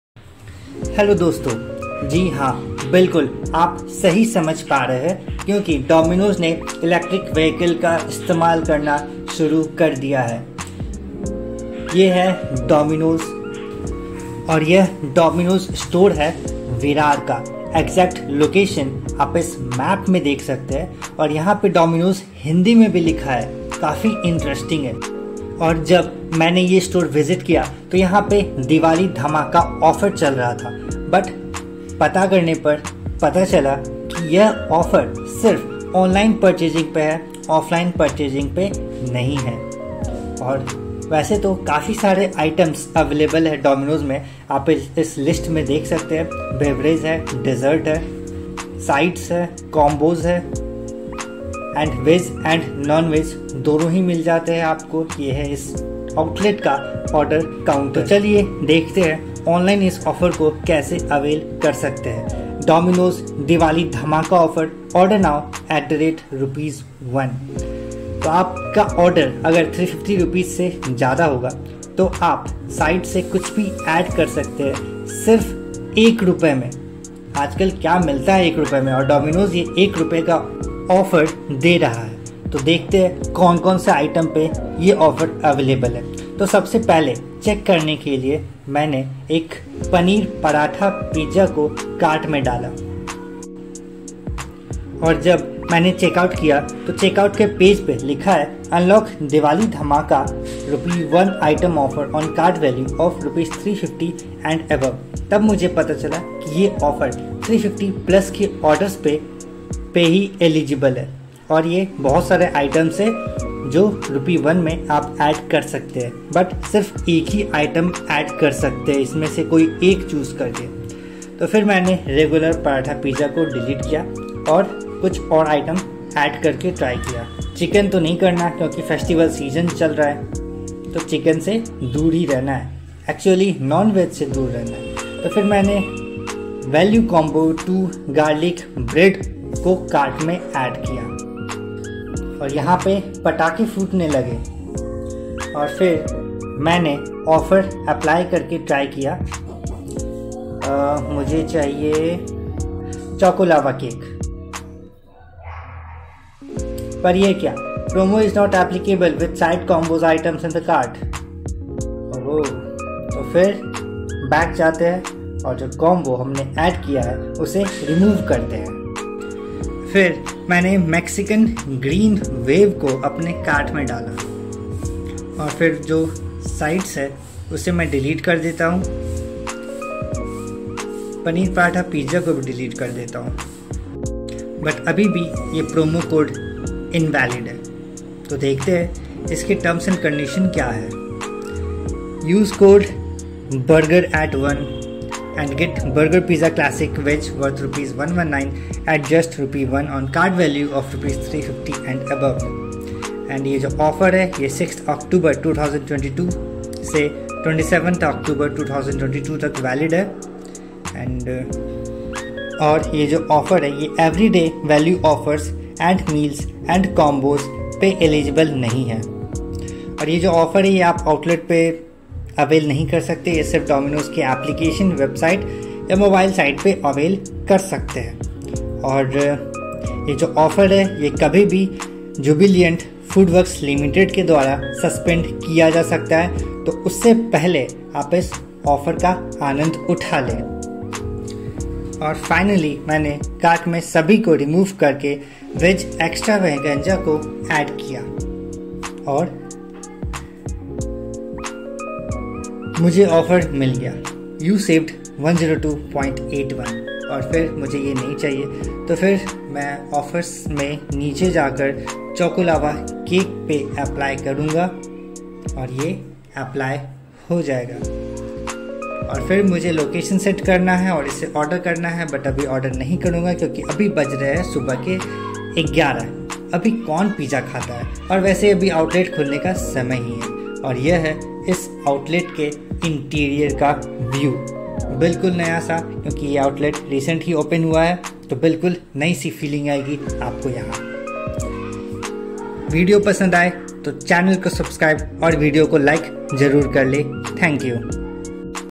Domino's Diwali Dhamaka @1₹ || 2022
Choose any 1 of the 9 flavourful food items for just ₹1 on a MOV* of ₹350 and above.

Using ₹1 never felt so good!